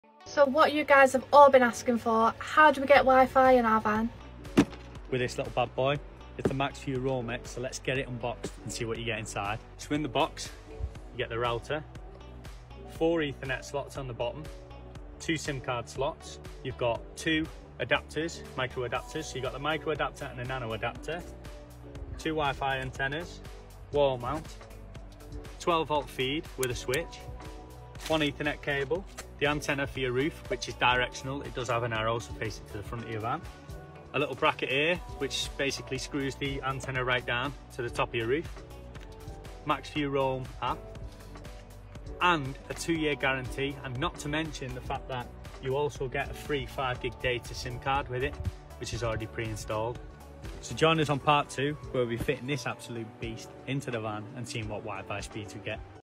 HOW WE GET WIFI (Van Life)
Hey guys! So as many of you already know we’ve been really struggling with internet whilst we’re on the road. (Living in a tin box doesn’t help) 🙈

The guys at @maxviewltd have very kindly sent us a Max View Roam X out to review.

We genuinely would never make videos on products we wouldn’t buy ourselves, so we have been testing this absolute gem out for a few weeks whilst we are in Scotland  and it’s a game changer!!

We can finally upload YouTube videos with ease and even jump on a TikTok live whilst we’re up in the highlands🙌🏽🎉

Here’s a video on everything that’s included in the box

Drop us a follow and stay updated, we will be showing you exactly how we installed the Roam X to our van and showing you real time speed tests in some of the most remote areas of Scotland! 🙌🏽

*the antenna is directional on the roof, but works omnidirectional*